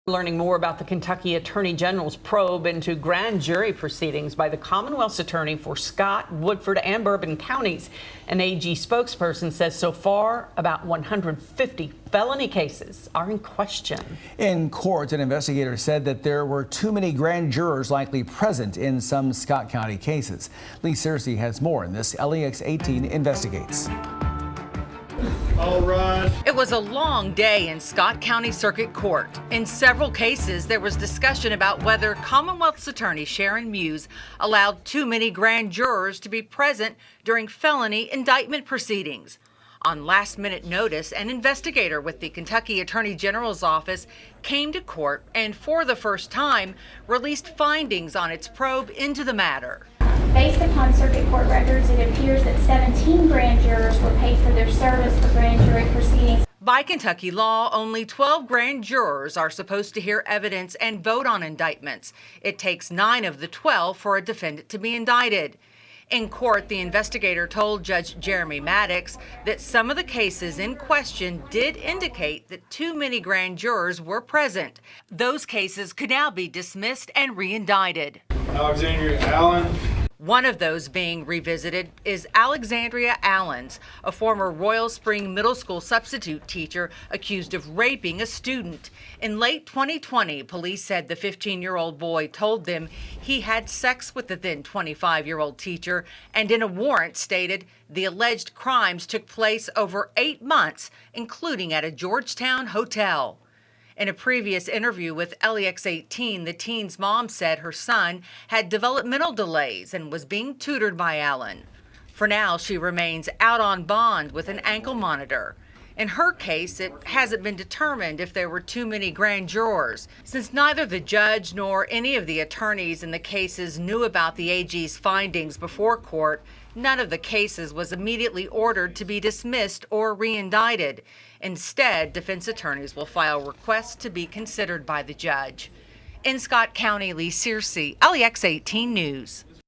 New information released on grand jury probe
An investigator with the Kentucky Attorney General’s Office was in Scott County Circuit Court Monday, releasing some of the first findings of the office’s probe into allegations involving grand jury hearings.